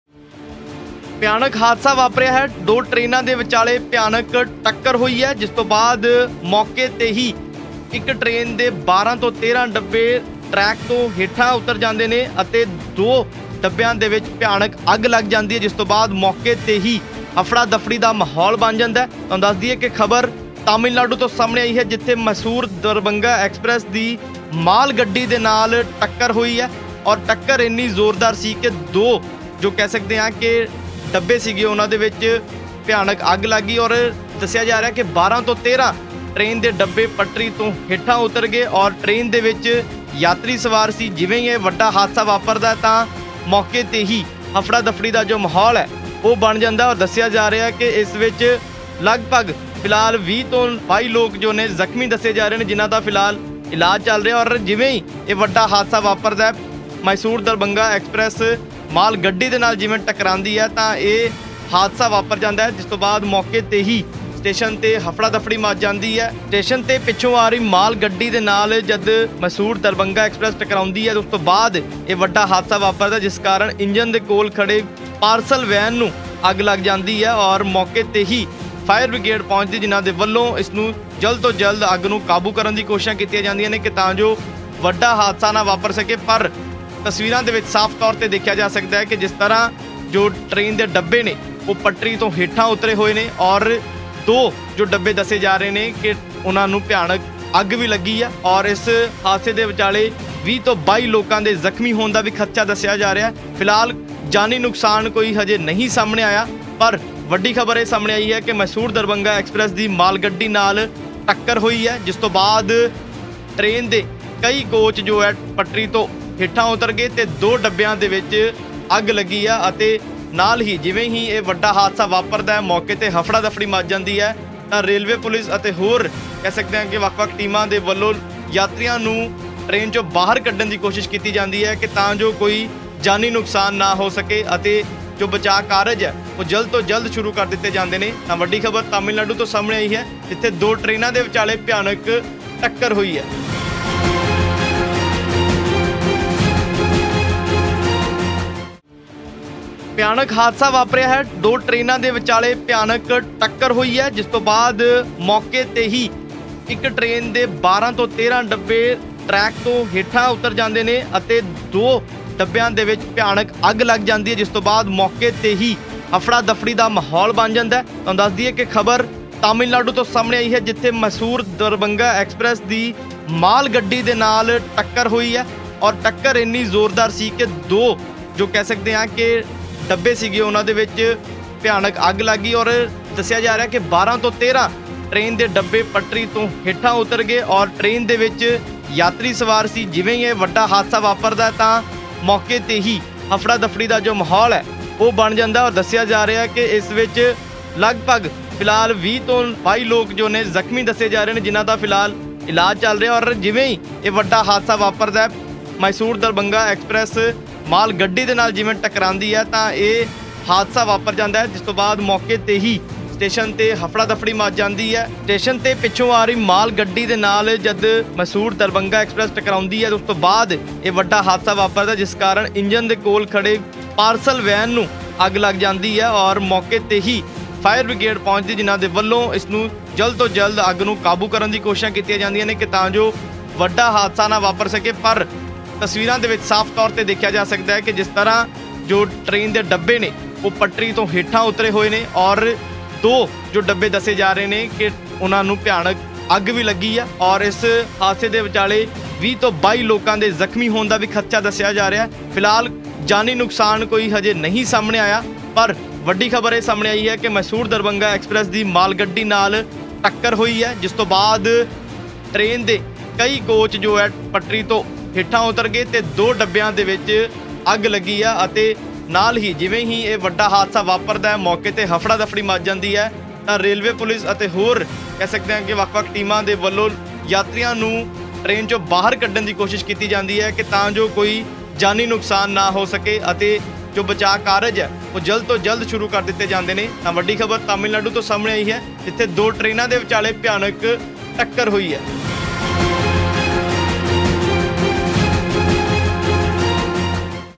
Tamil Nadu train collision: ਮਾਲਗੱਡੀ ਨਾਲ ਟਕਰਾਈ Mysuru-Darbhanga Express, ਟਰੇਨ 'ਚ ਲੱਗੀ ਅੱਗ | N18V
ਮਾਲਗੱਡੀ ਨਾਲ ਟਕਰਾਈ
ਮੈਸੂਰ-ਦਰਭੰਗਾ ਐਕਸਪ੍ਰੈਸ
ਟਰੇਨ 'ਚ ਲੱਗੀ ਅੱਗ
ਪਟੜੀ ਤੋਂ ਉੱਤਰੇ 13 ਕੋਚ